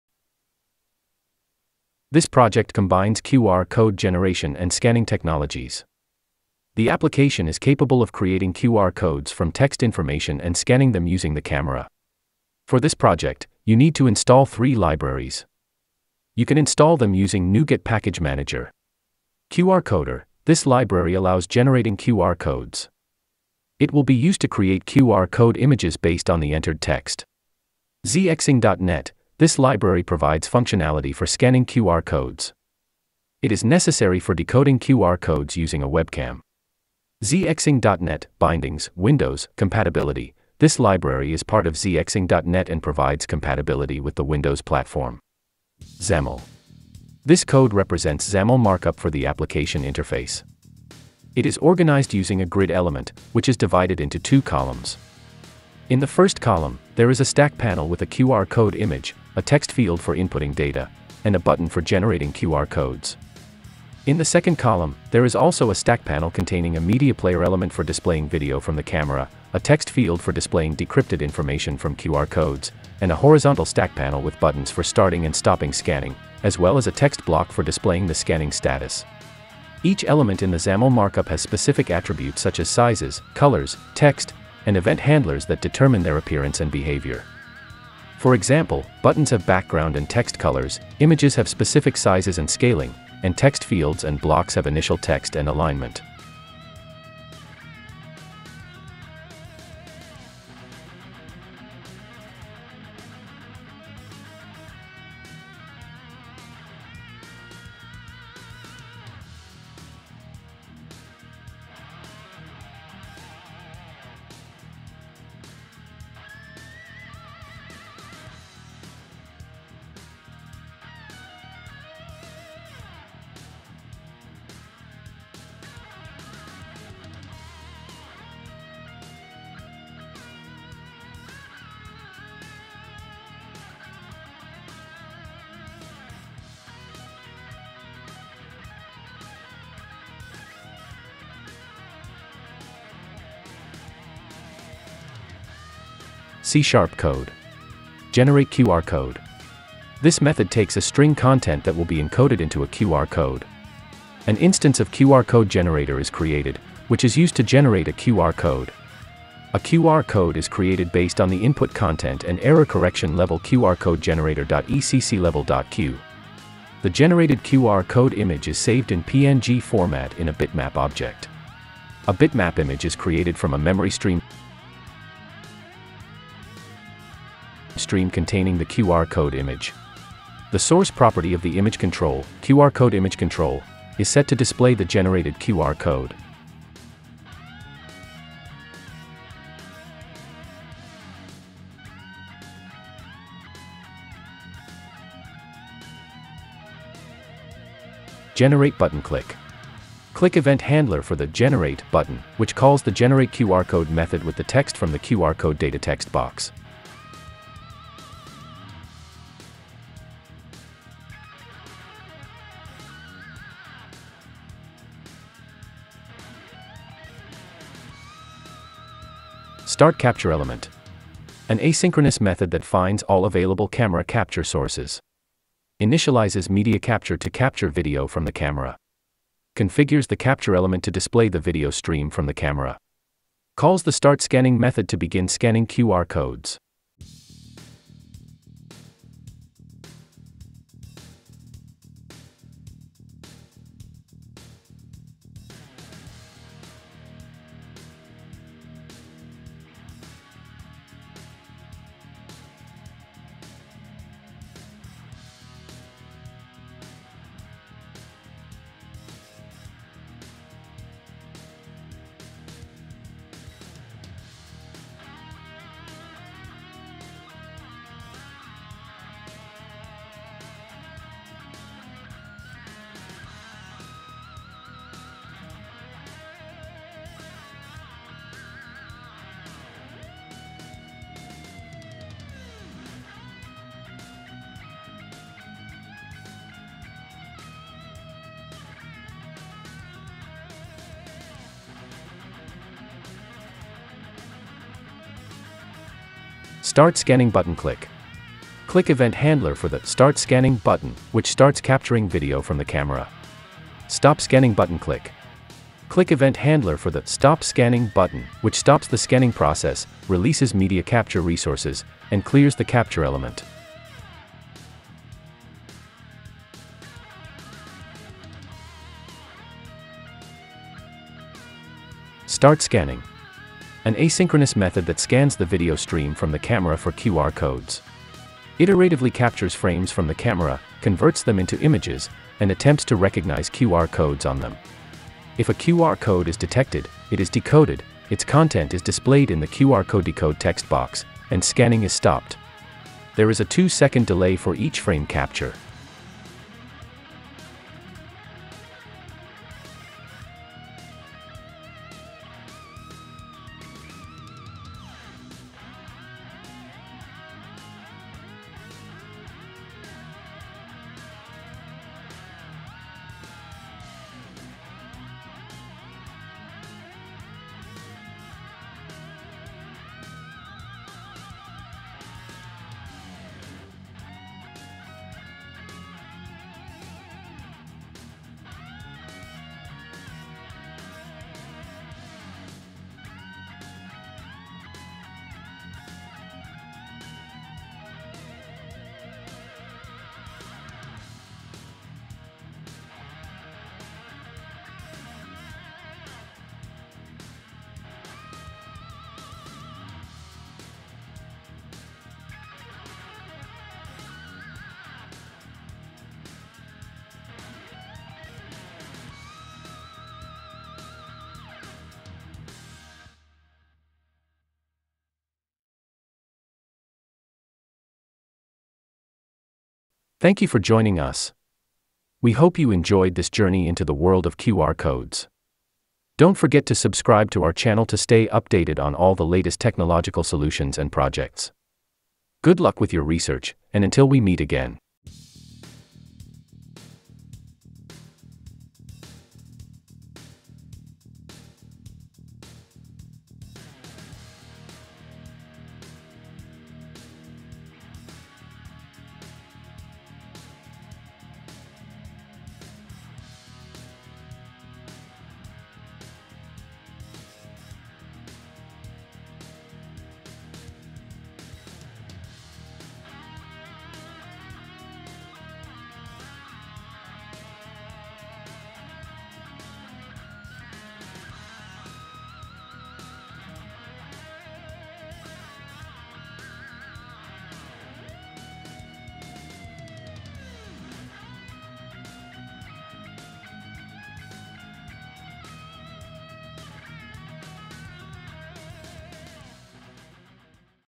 How to Generate and Scan QR Codes with Webcam Using C# in WinUI
In this video tutorial, we delve into the integration of QR code generation and scanning functionalities using C# in WinUI. Learn how to seamlessly create QR codes from textual information and effortlessly scan them using your webcam.